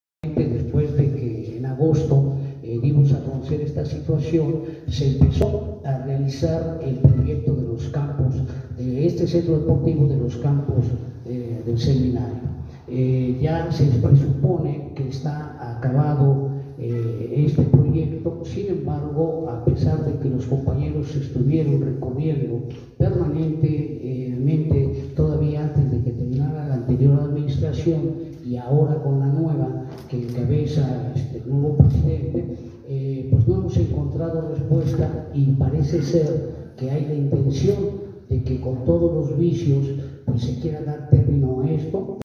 Solicitan apoyo al Gobierno Municipal para la preservación de los campos del seminario
Solicitan apoyo al Gobierno Municipal para la preservación de los campos del seminario y la construcción de un centro deportivo, integrantes del colectivo Campos Verdes Unidos y Colectivo Campos del Seminario.